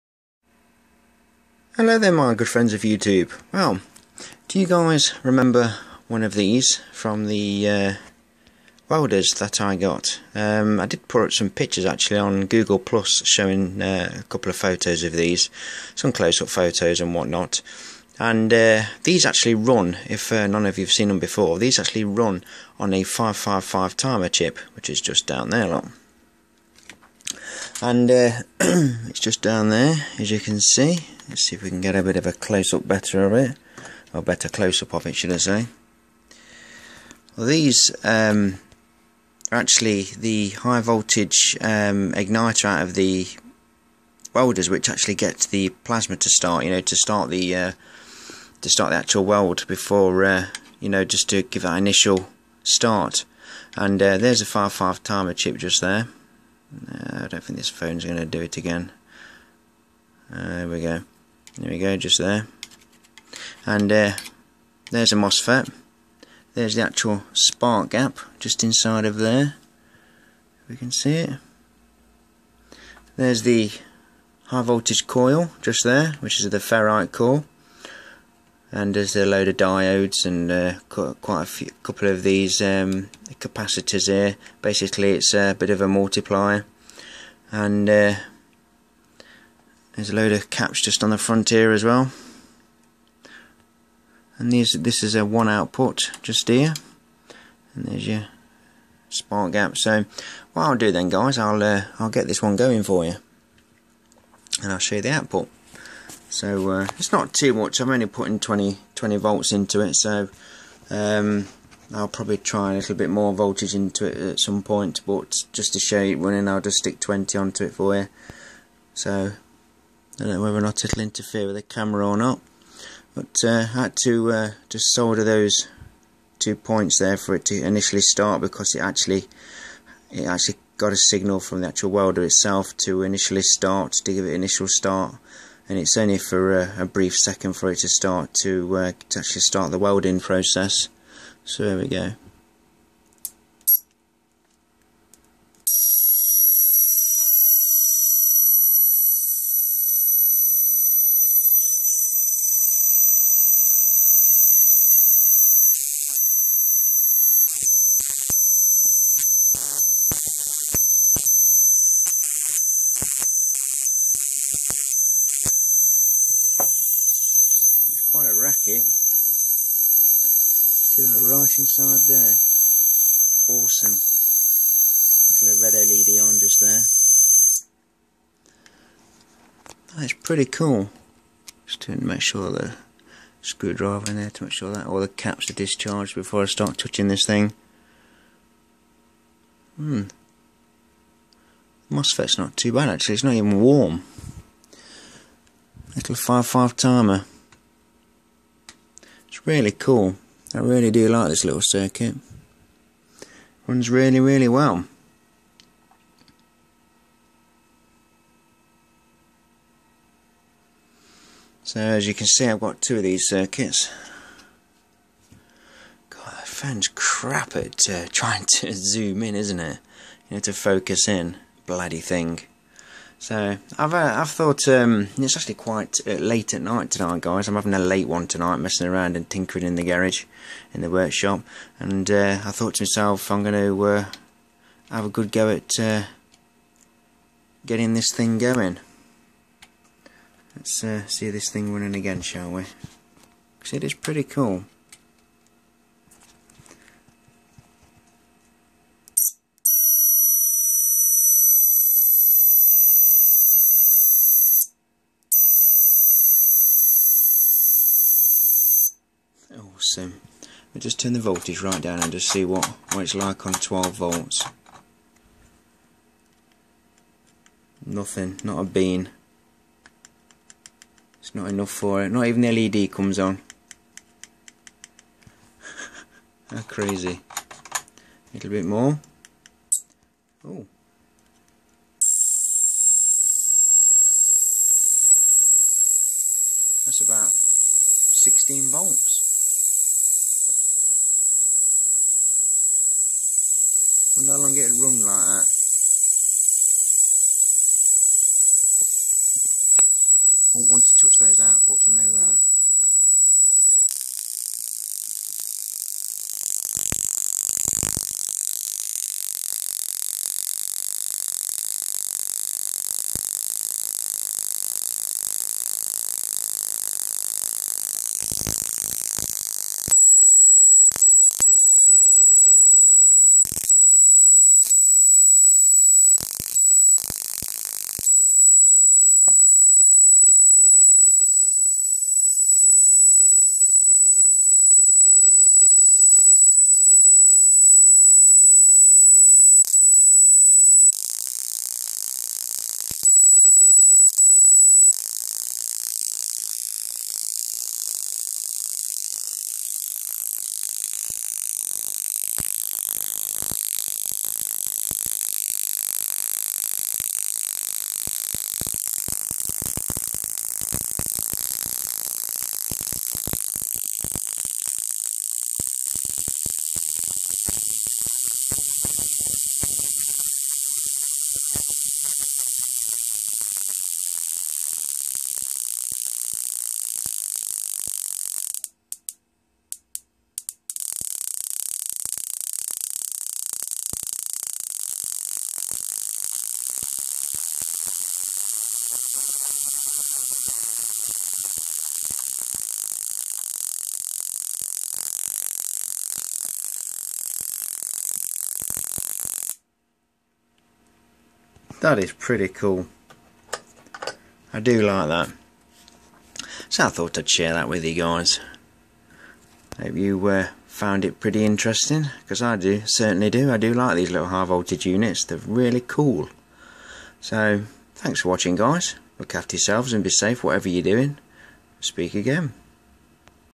High voltage unit' spark gap (ear protection needed)
High guys this is a 6 kv high voltage unit out of a welder.  And I got it working ;) (please be aware high frequencys) 😵

Hope ya enjoy and thanks friends for watching ;-)